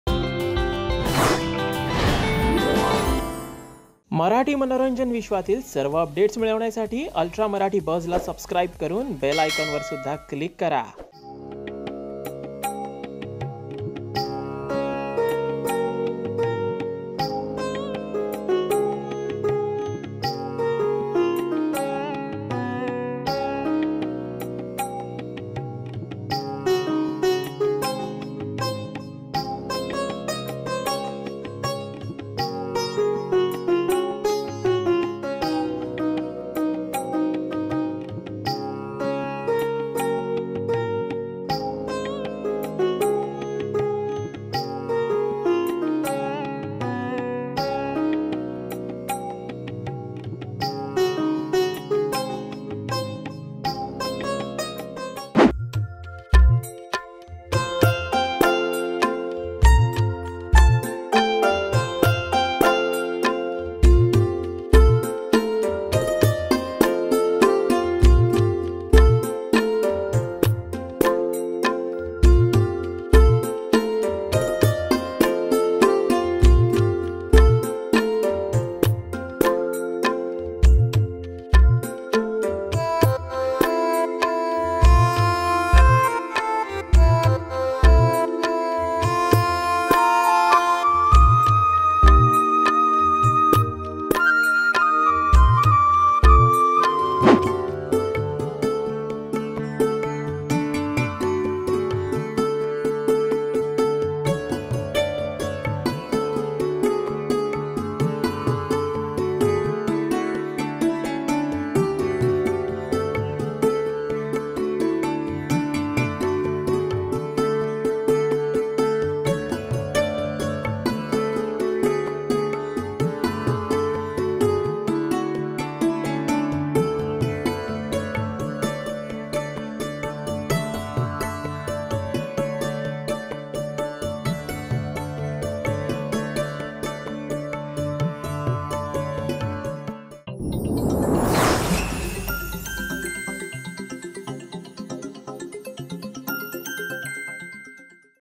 प्राजक्ता गायकवाडच्या घरी गौरी पूजन | Swarajya Rakshak Sambhaji | Marathi Actress | Gauri Pujan 2021
प्राजक्ता गायकवाडच्या घरी गौरी पूजन | आई माझी काळूबाई | Swarajya Rakshak Sambhaji | Marathi Actress | Gauri Pujan 2021

अभिनेत्री "प्राजक्ता गायकवाड"नं गौरी पूजनावेळी केलेलं नृत्य पाहा "अल्ट्रा मराठी बज"वर ! Report By Sagar Maladkar Video Editor - Sushant Varsolkar